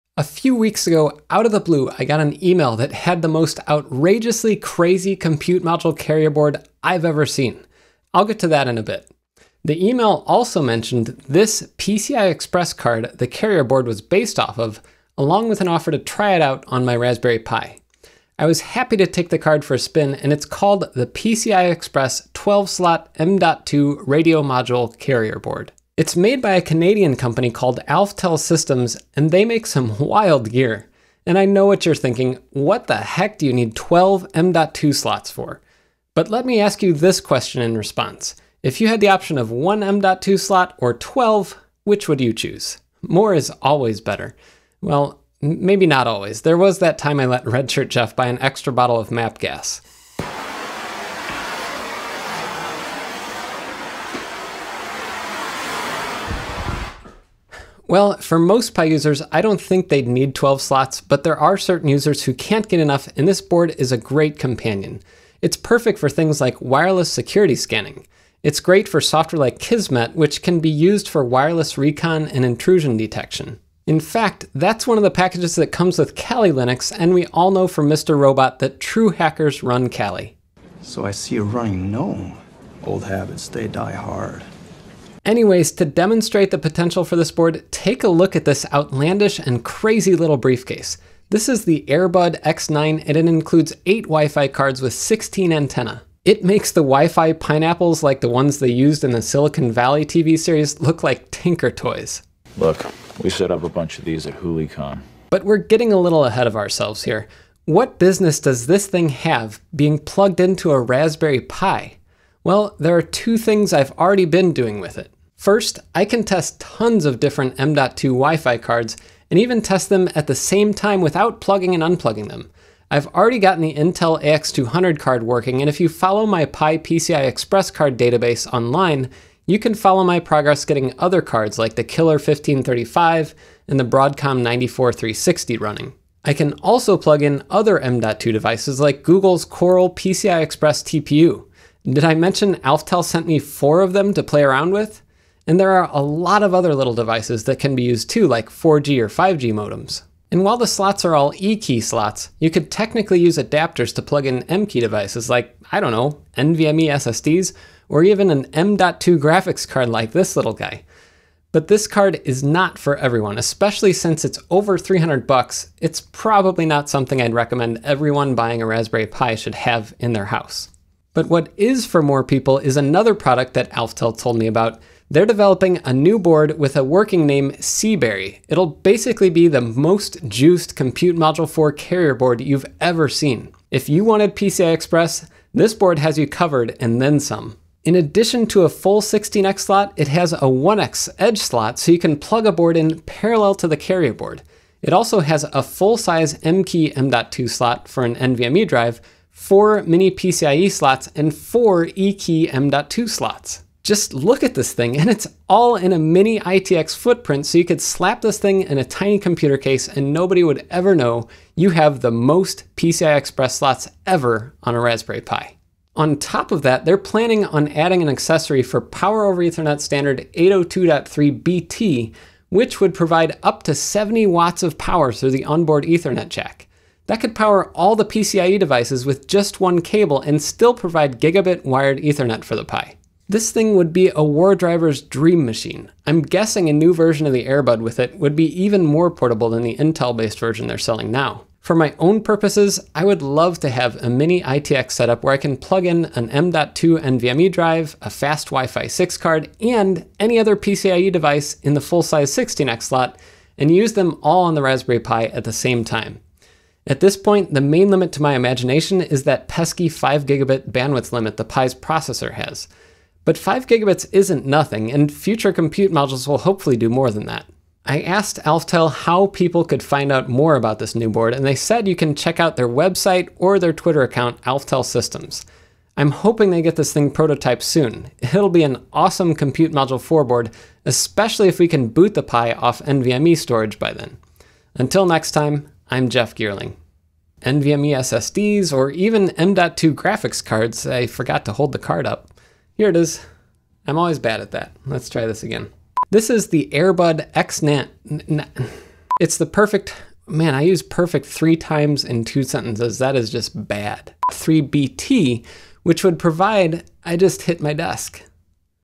12 PCI Express M.2 Slots on a Raspberry Pi!?
I take Alftel Systems' 12-slot PCI Express M.2 carrier board for a spin on my Raspberry Pi Compute Module 4, and mention their soon-to-be-announced 'Seaberry' Mini ITX board for the Compute Module!

Follow all the PCI Express cards I'm testing with the Compute Module 4